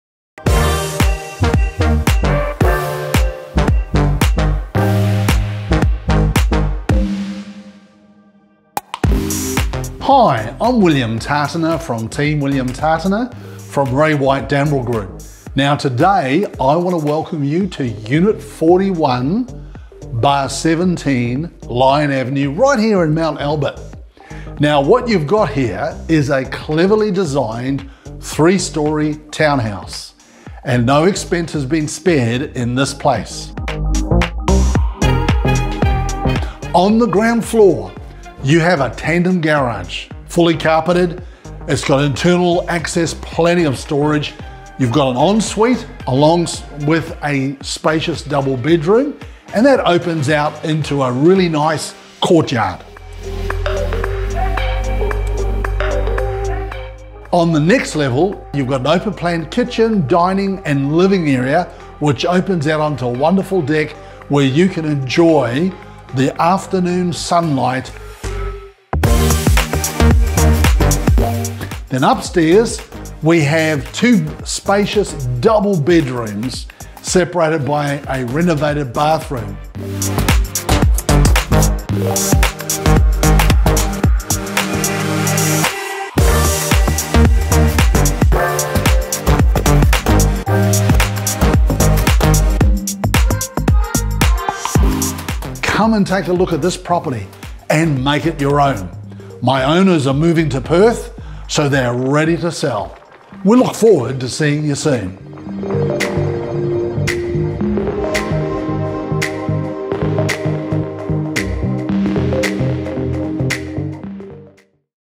For Sale | 41/17 Lyon Avenue, Mount Albert
This immaculately presented terrace minimal maintenance townhouse, provides a comfortable lifestyle for buyers of all levels that will find this home spacious and fitted out with extra quality fixtures.